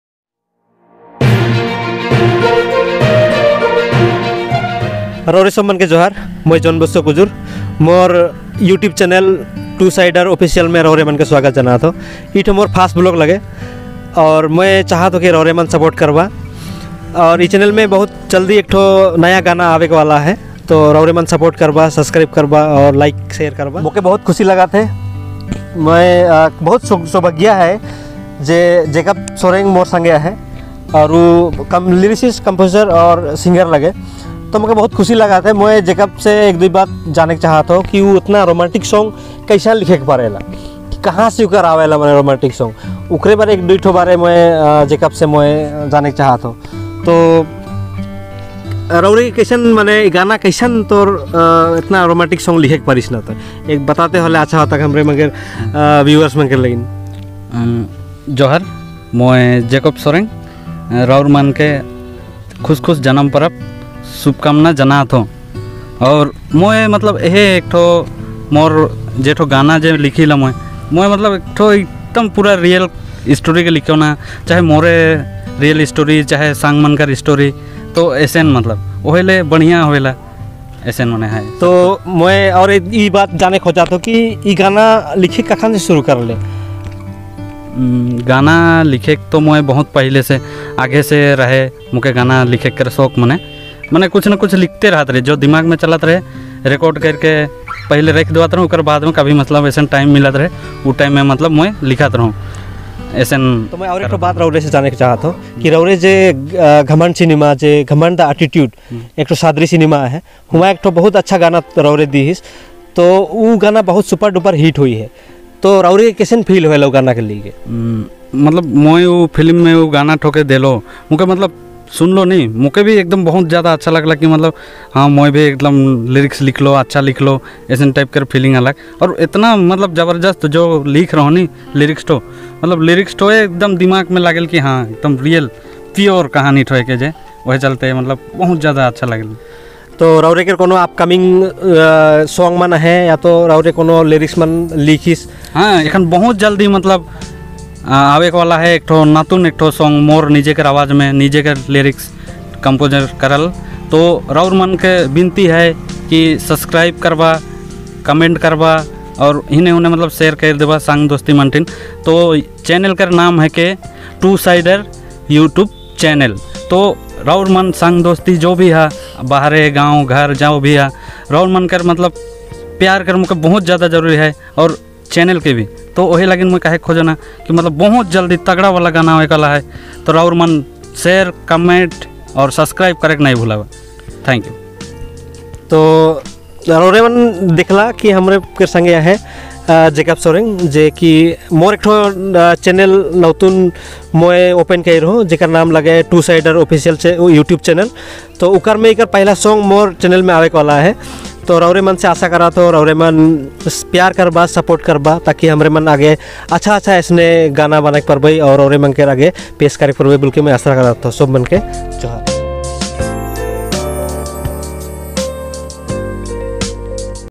My First Vlog with Jacob Soreng || 2Sider Official // John Bosco Kujur //
My First Vlog with Jacob Soreng at Doomni Tea Estate.

Johar Friends,

Special thanks to John Bando (John's DigiHub) and to Sanjay Panna (Panna's Vlog) for your support.

Regards,
John Bosco Kujur